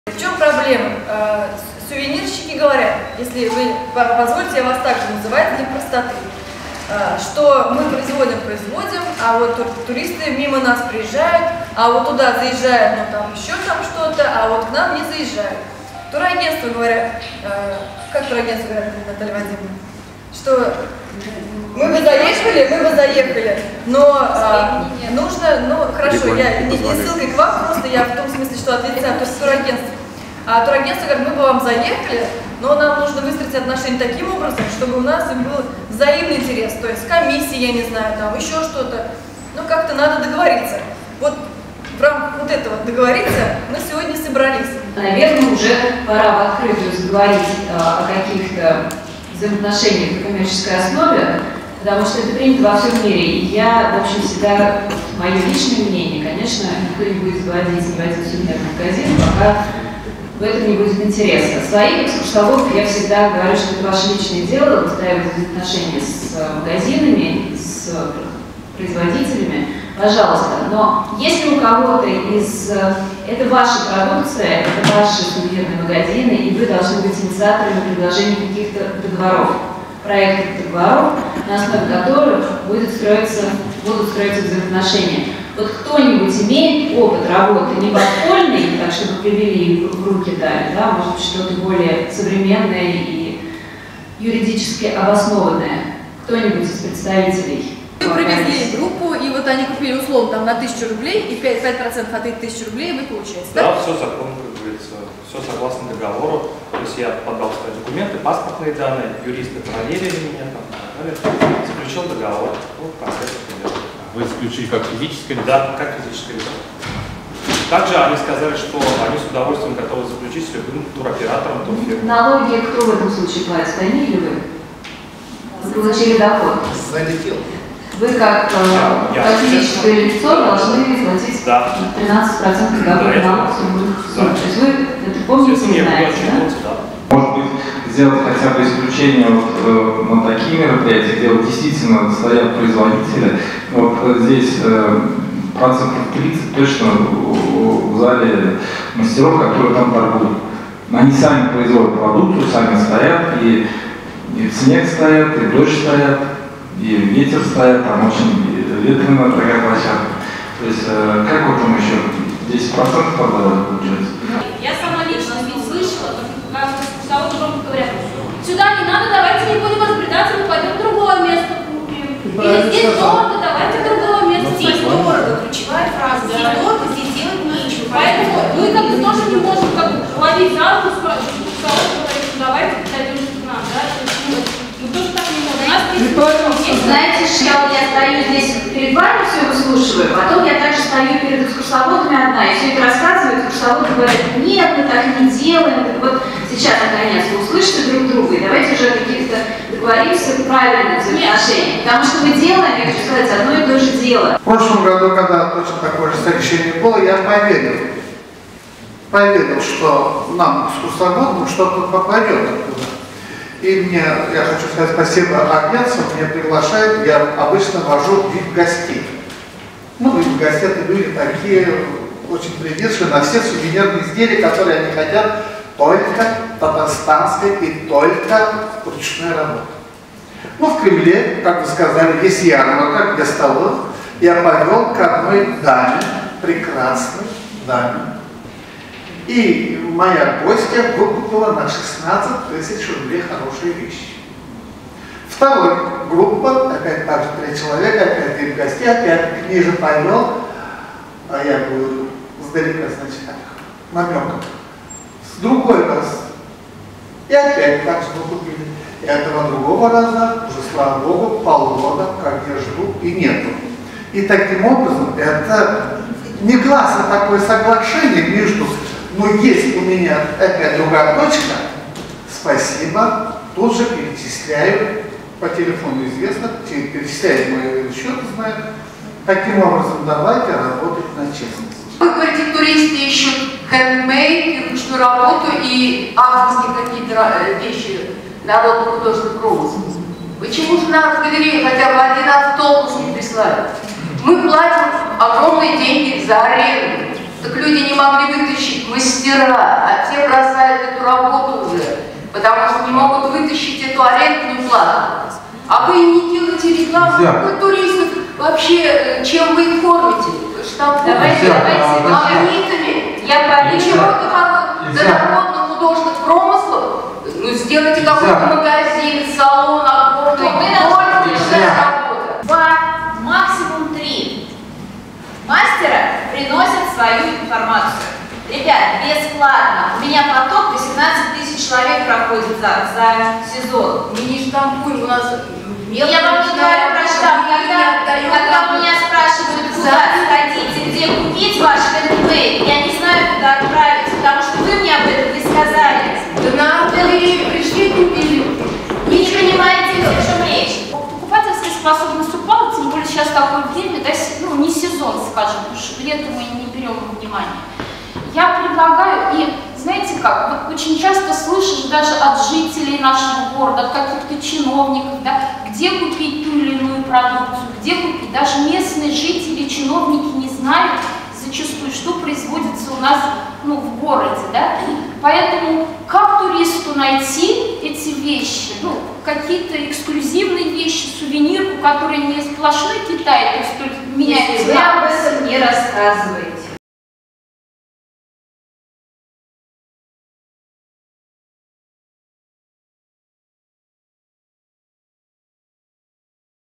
Спор предпринимателей с чиновниками возник в ходе обсуждения развития сувенирки в Казани
Эмоционально и на повышенных тонах прошла вчера в Торгово-промышленной палате РТ встреча представителей туристического бизнеса республики. Корреспондент «БИЗНЕС Online», побывавшая на встрече, услышала, как сувенирщики жалуются на то, что туристы обходят их стороной, представители туркомпаний — на незаинтересованность продавцов сувениров в предоставлении скидок, а представители комитета по туризму — на пассивное выжидание сувенирщиков и нежелание рекламировать собственную продукцию